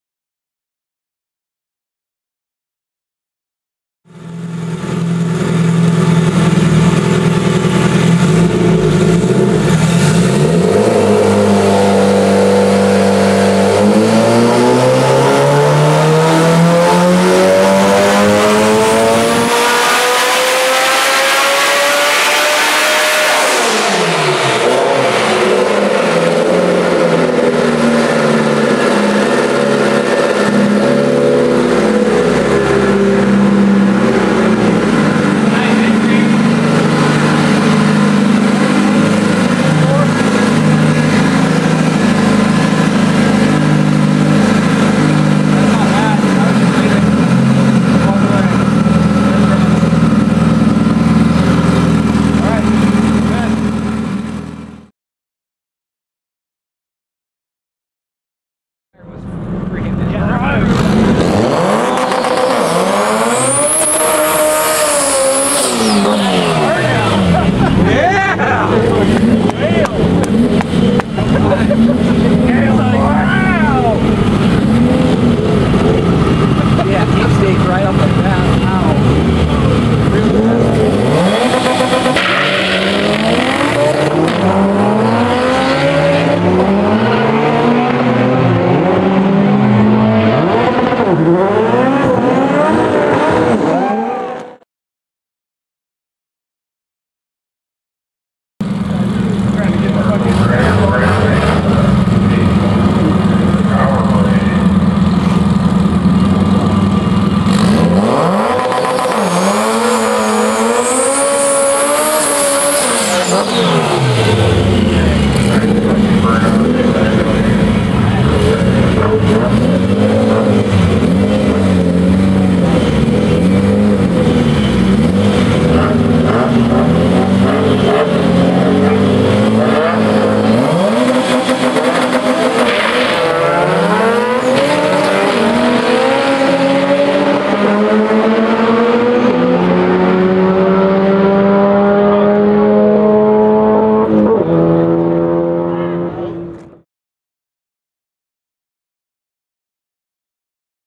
SpeedFactory 2010 Sleeved D2B Vitara D16 - New power & ET records
Record dyno pull, and first runs with the setup.